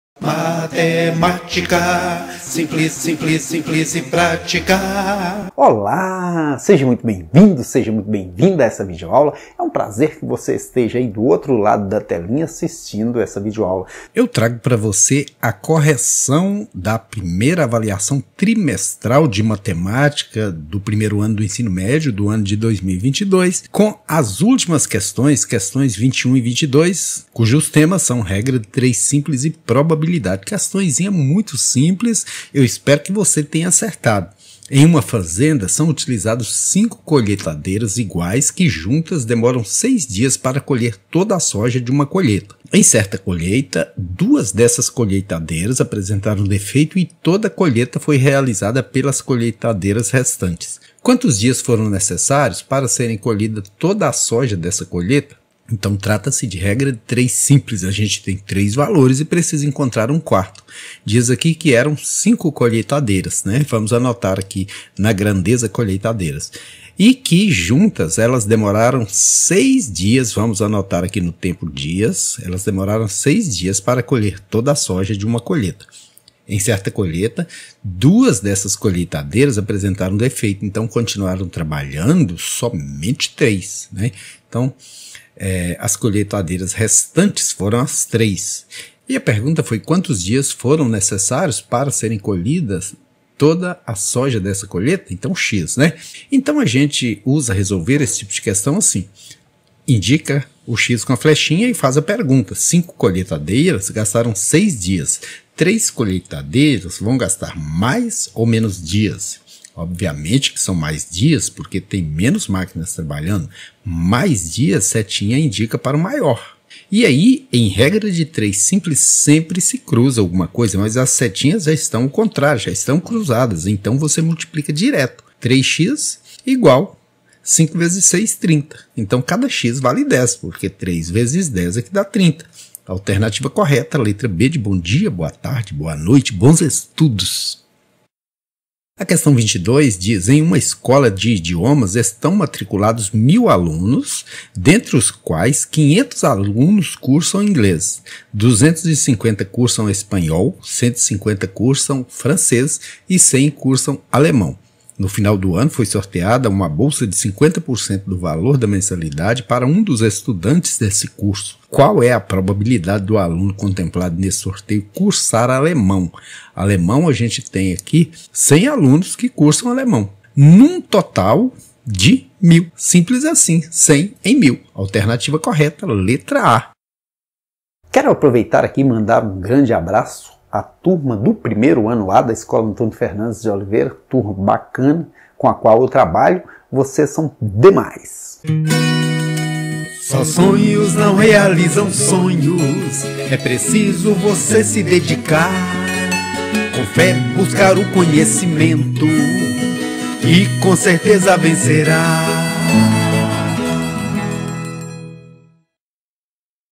CORREÇÃO DA 1ª AVALIAÇÃO TRIMESTRAL, 1º ANO (Questões 21 e 22, Matemática)
CORREÇÃO DA 1ª AVALIAÇÃO TRIMESTRAL, 1º ANO (Questões 21 e 22, Matemática)Geometria espacial ,Sistema de equações
do 1º grau, Área de figuras planas